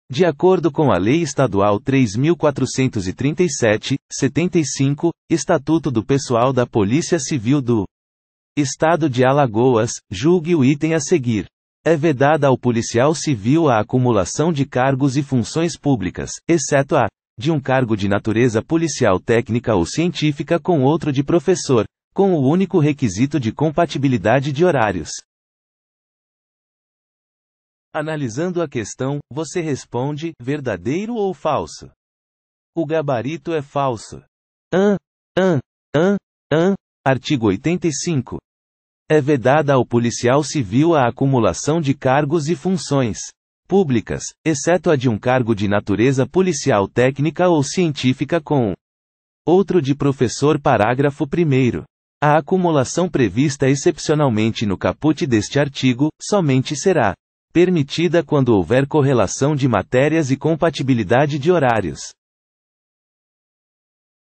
QScon Direito: [Art. 85, Lei Estadual 3.437/75 - AL] Texto de Lei
Art. 85, Lei Estadual 3.437/75 AL | Texto de Lei

De acordo com a Lei Estadual 3.437/75 (Estatuto do Pessoal da Polícia Civil do. Estado de Alagoas), julgue o item a seguir. É vedada ao policial civil a acumulação de cargos e funções públicas, exceto a. de um cargo de natureza policial técnica ou científica com outro de professor,. com o único requisito de compatibilidade de horários.

Gabarito: Falso